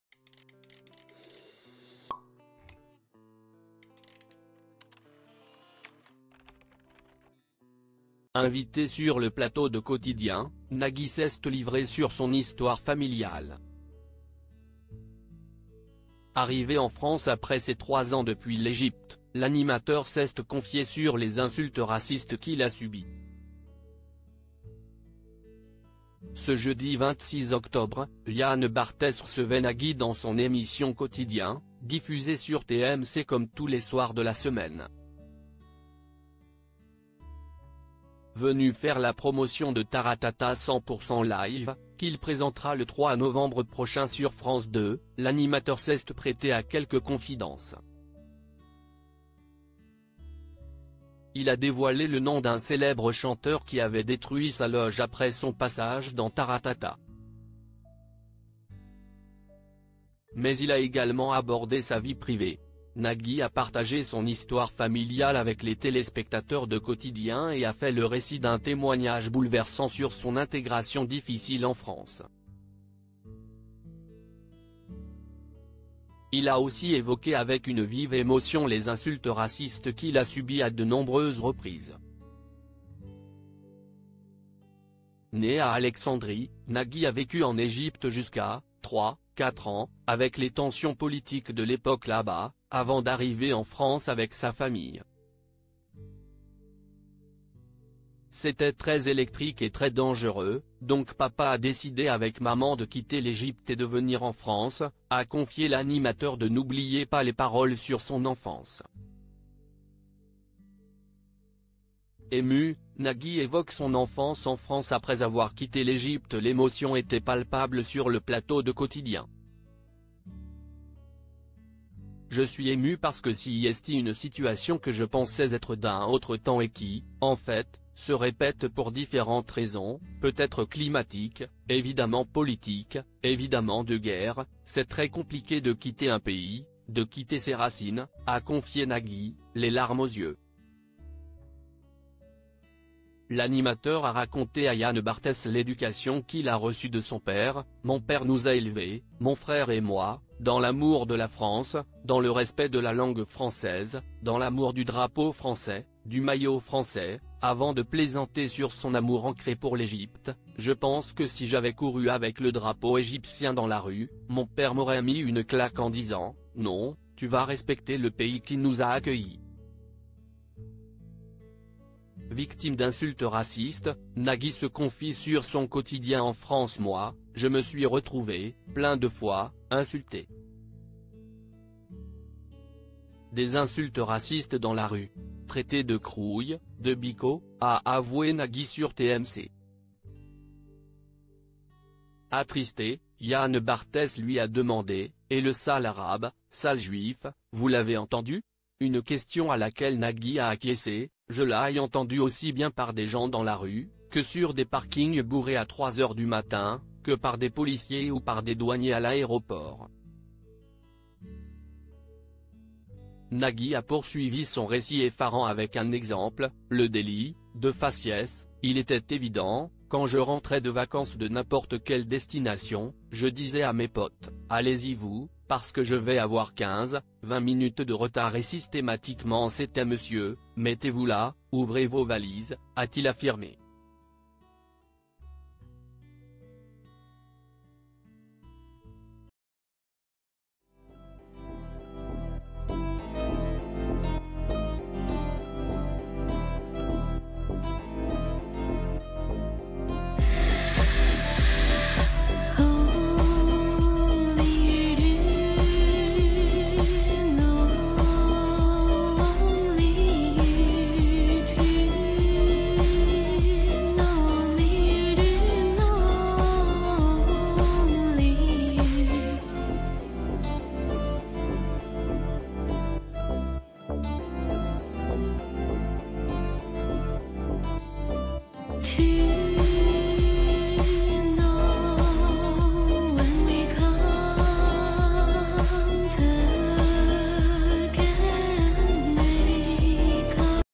Je ne pensais pas parler de ça ému, Nagui se confie sur les insultes racistes qu'il a subies
"Je ne pensais pas parler de ça..." : ému, Nagui se confie sur les insultes racistes qu'il a subies et évoque son histoire familiale